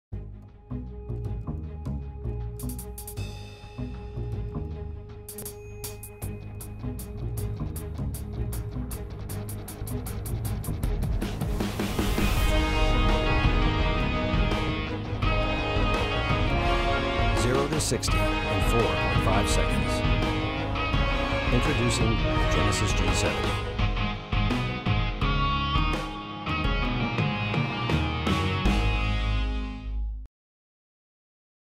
NARIP CAR ADVERT Oct 31, 2018 1
I composed the music for this is a 30-second DEMO ONLY. Genesis Motors. Genesis Motors is the luxury vehicle division of the South Korean vehicle manufacturer Hyundai Motor Group. Initially envisioned along with the plan for Hyundai's new luxury sedan Hyundai Genesis in 2004, the Genesis brand was officially announced as a standalone marque on 4 November 2015.